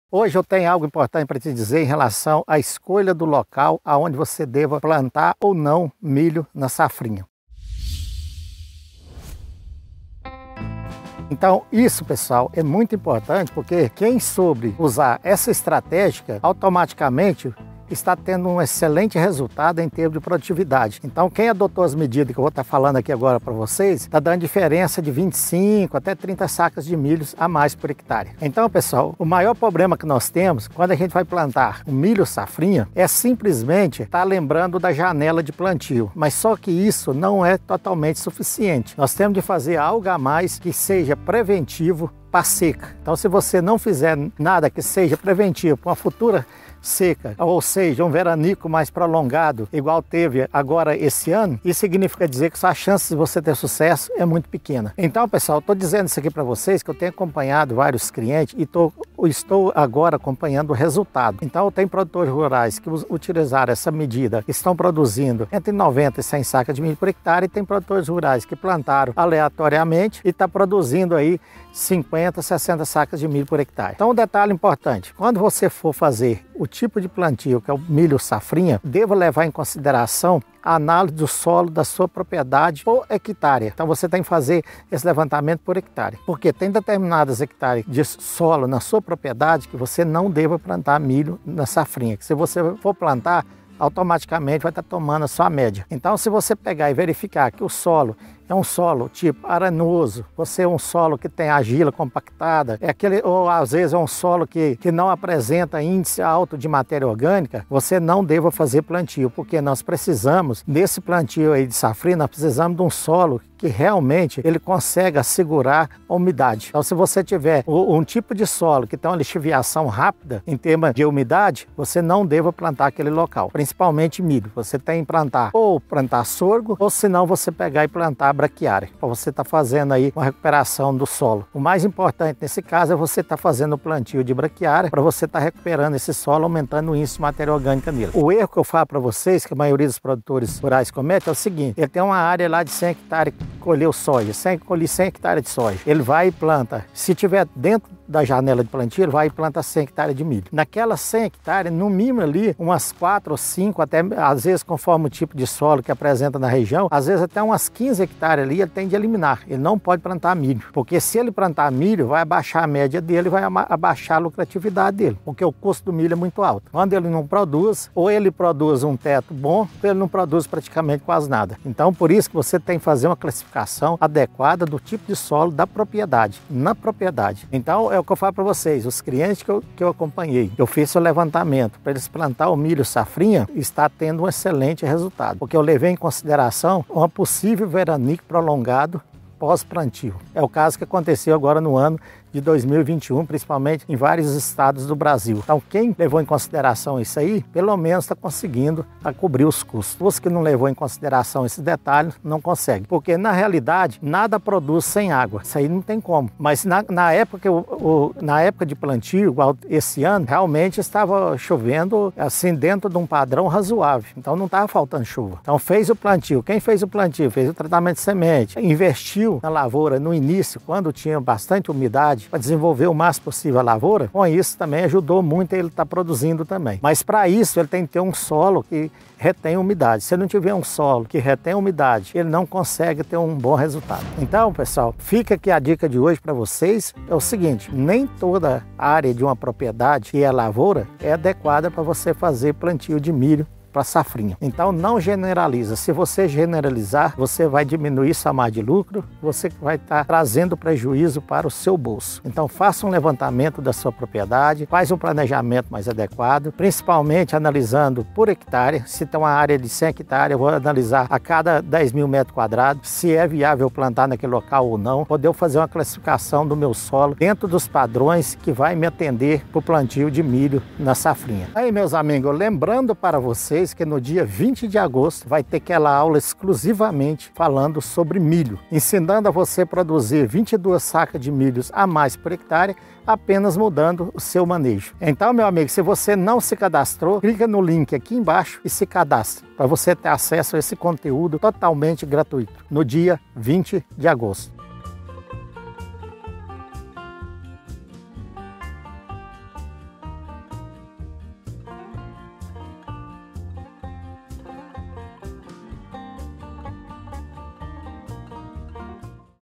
COMO ESCOLHER A MELHOR ÁREA DE PLANTIO PARA O MILHO NA SECA
Não é segredo pra ninguém que a seca desse ano prejudicou muito os produtores rurais que foram pegos de surpresa e não souberam o manejo correto para se prevenir contra os danos climáticos causados por ela.
Nesse vídeo eu te mostro como escolher a melhor área de plantio para o milho, para que você consiga ter um bom resultado mesmo com uma seca inesperada, como foi essa dos últimos meses. Espero que este conteúdo te ajude, um abraço do Aminter!